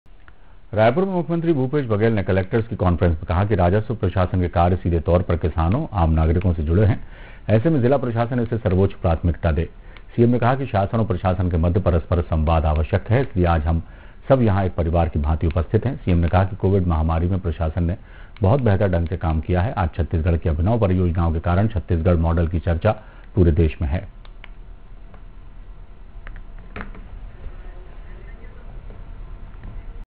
कलेक्टर्स के साथ कॉन्फ्रेंस में कानून व्यवस्था पर बोले सीएम भूपेश बघेल-News World
कलेक्टर्स के साथ कॉन्फ्रेंस में कानून व्यवस्था पर  बोले सीएम भूपेश बघेल